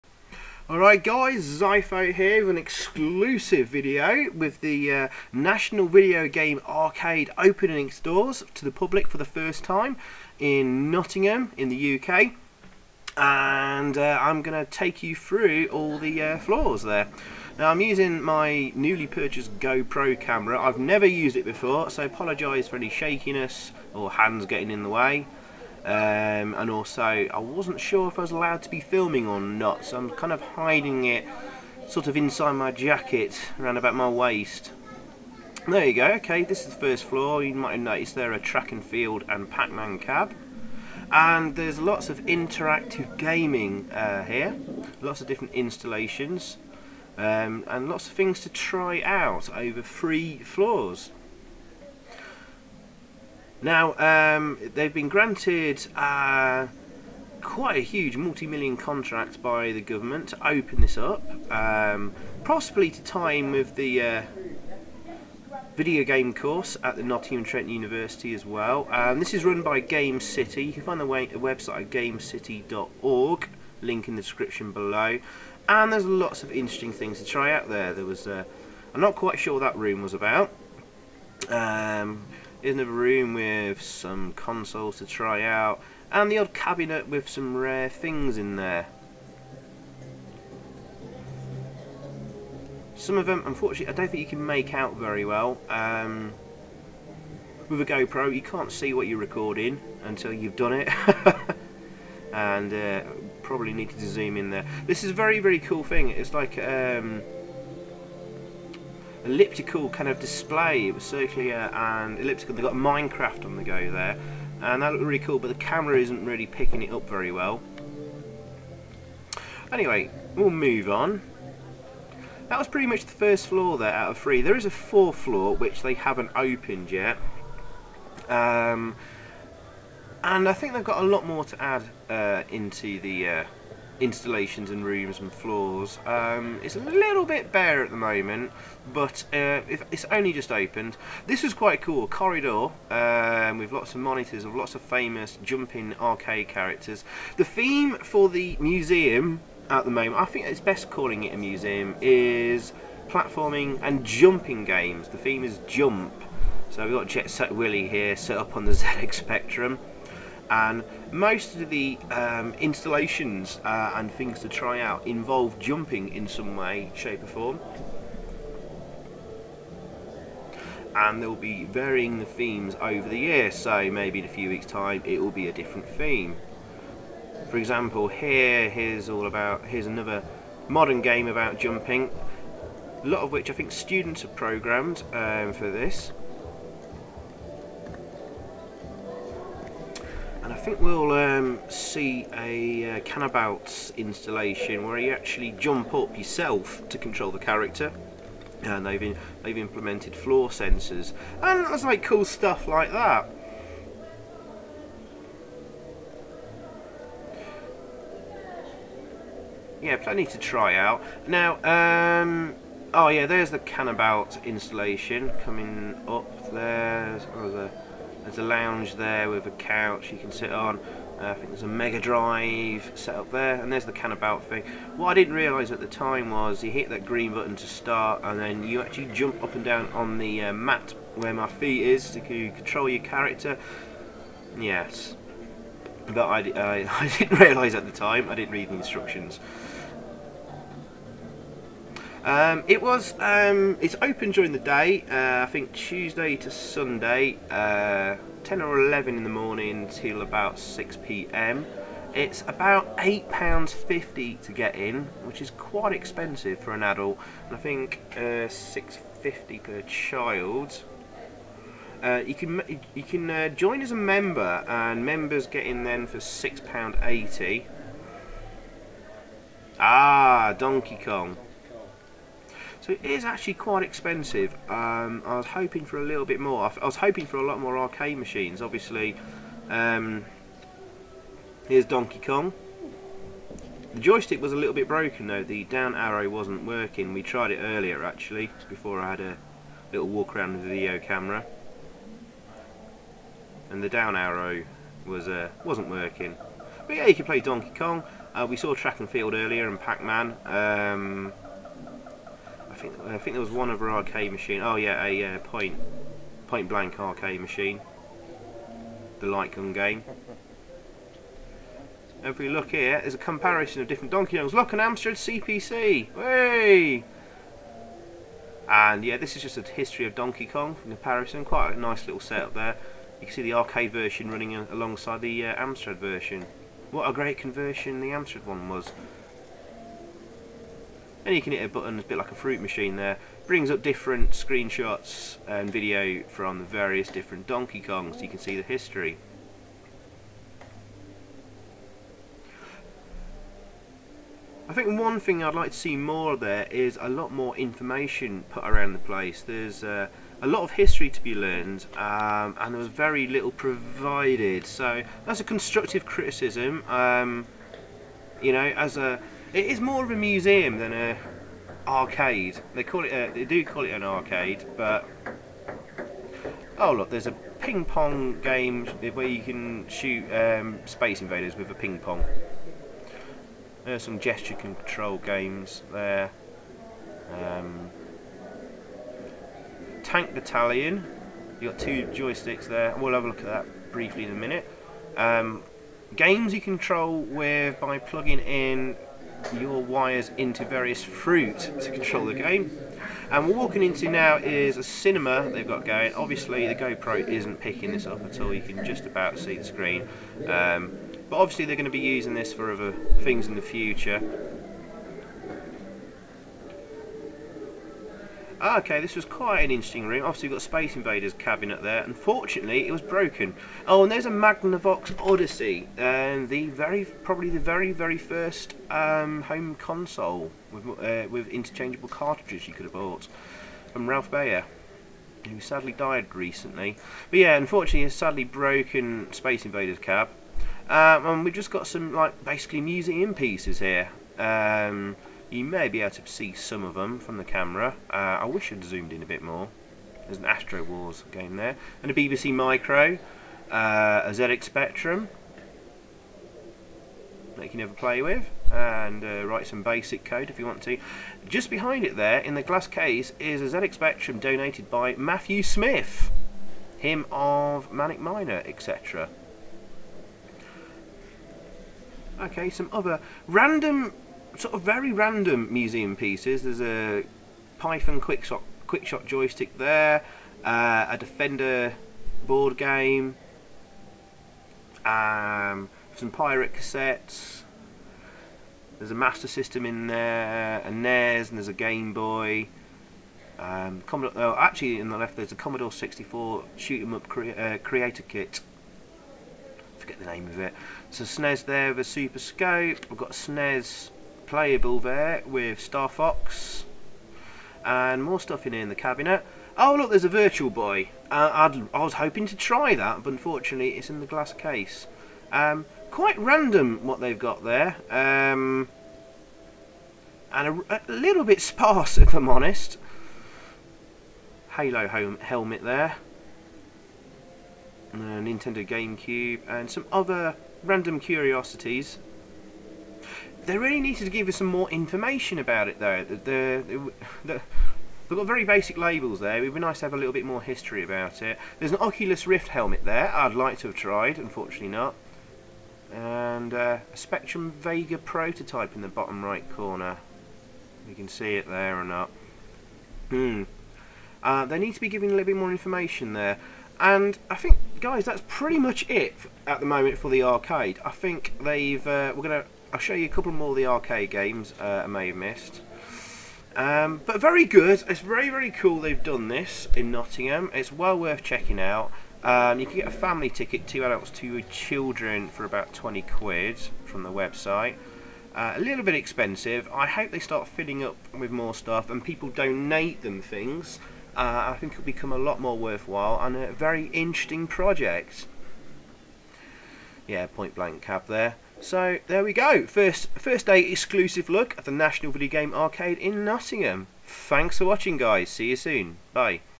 National Videogame Arcade (Nottingham) - Opening Day Exclusive Look Around!
Opened to the public on Saturday 28th March is the UK's first video game experience and museum! The "National Videogame Arcade" in Nottingham is - "the world’s first permanent centre to celebrate and explore videogame culture, in the heart of Nottingham City’s Creative Quarter."

Featuring arcade games, exhibitions, interactive displays and games, museum pieces, cinema, cafe, bar, and special evening events.. it's truly remarkable this exists!

Whilst it's still early days, a little expensive and I hope a lot more gets added to it (especially far more arcade machines - it's more of a museum and exhibition than an 'Arcade') it's certainly been good fun and very promising. Definitely worth at least a visit!

(Oh, and as I said in the video - first time using the GoPro and not used to it! Apologies if you can't make anything out, a little shaky at time, etc - also wasn't sure if I would be allowed to film or not so was trying to hide it away as best I can!!)